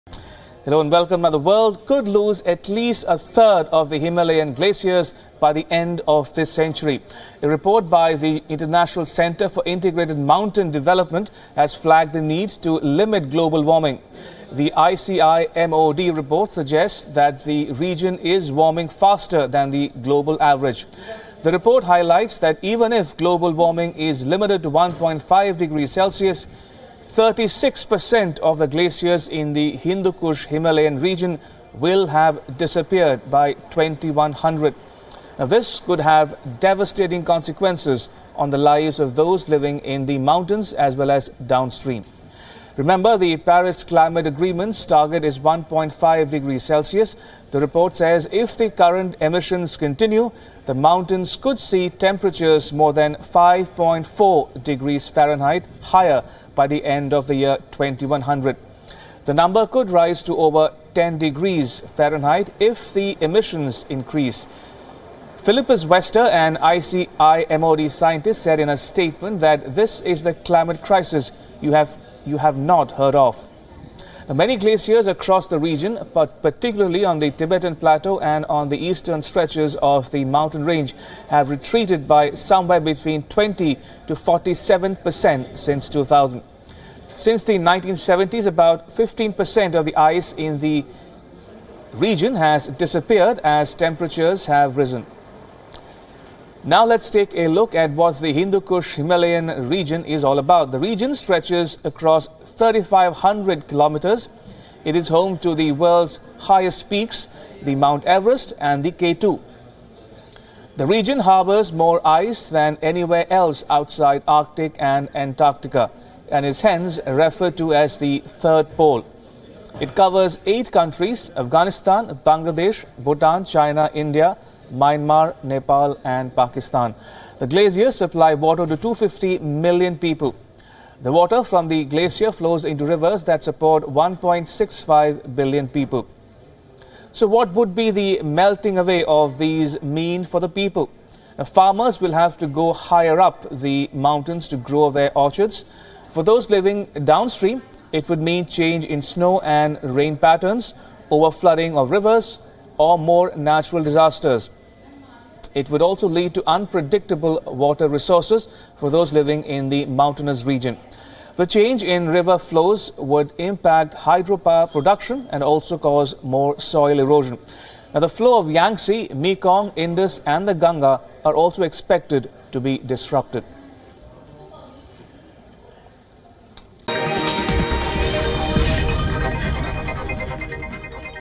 One-third of Himalayan glaciers will melt by 2100
At least a third of glaciers in the Hindu Kush-Himalayan range will melt away this century due to climate change, warns a new study.